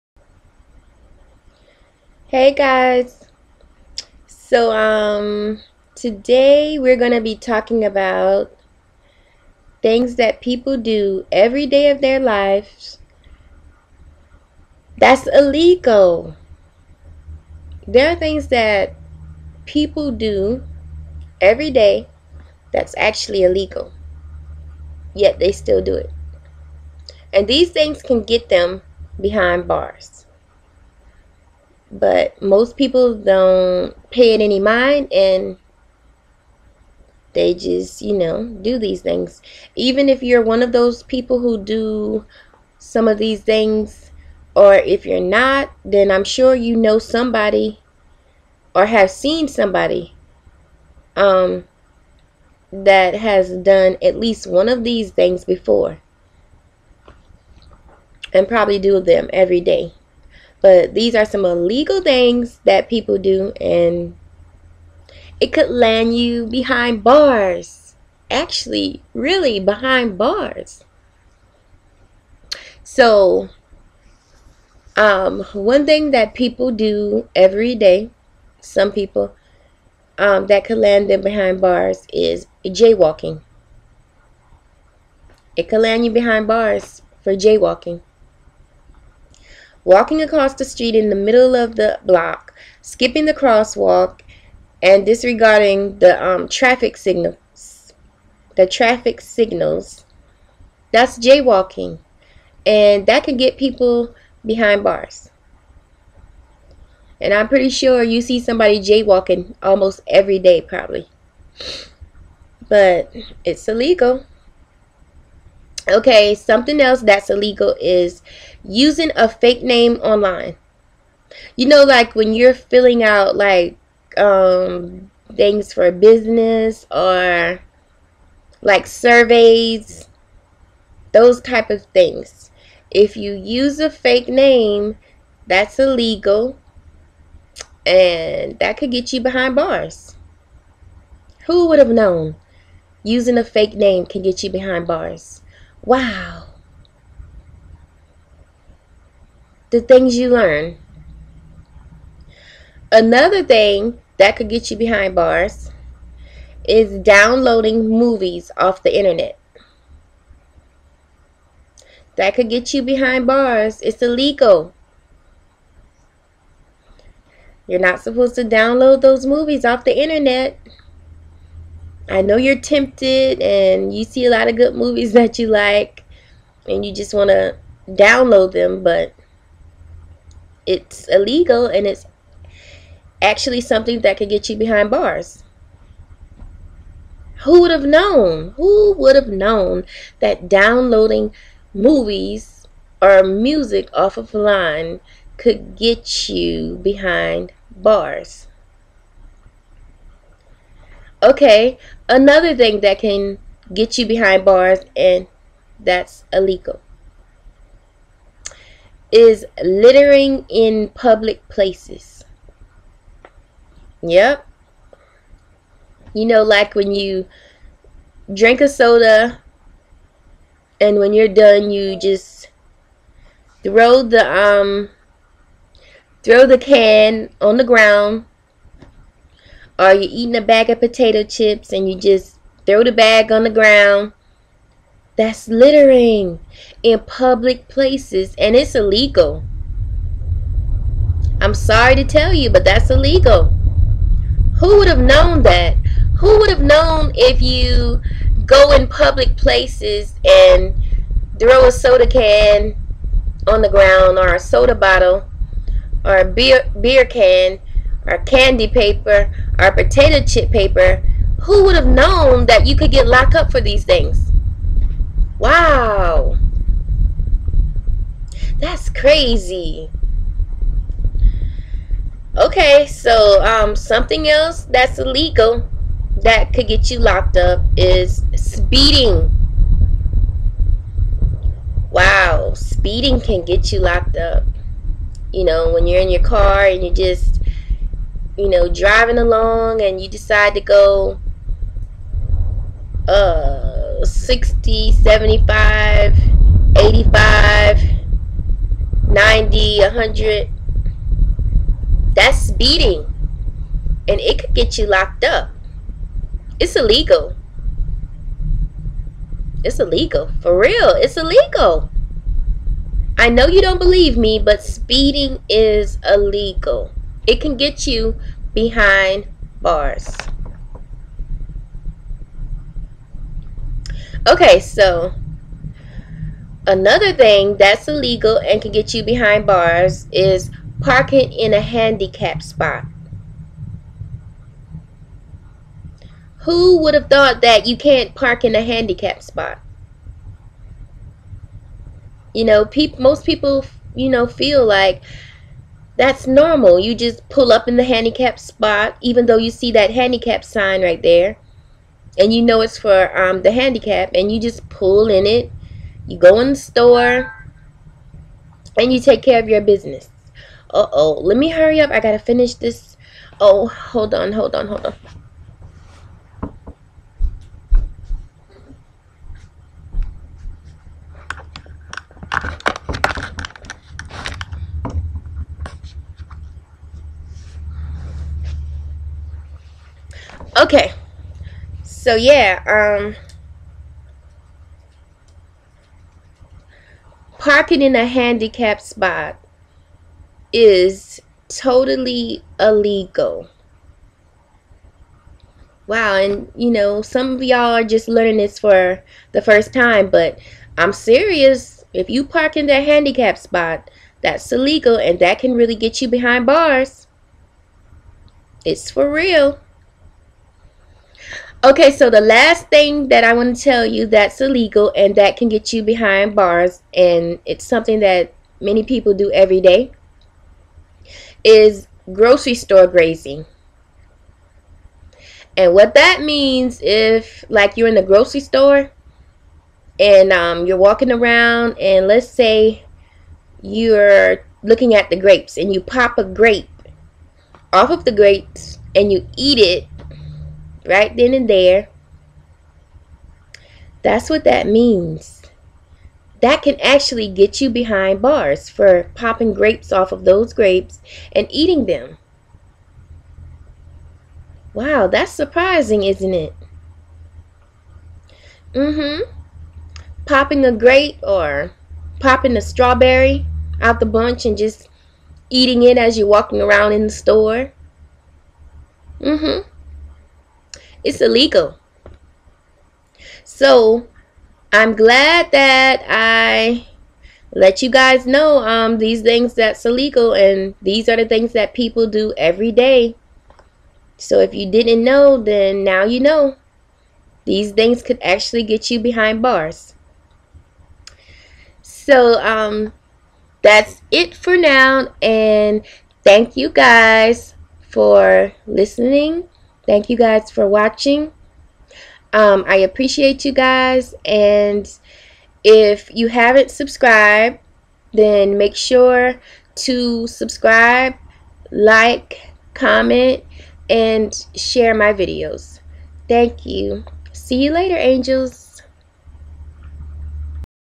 Illegal things you do everyday!!!
Model Life

Hi Angels these are illegal things that people do everyday...Hope you guys are starting the New Year out good... Don't forget to subscribe like comment n share. Follow me on facebook: Unique Mystery.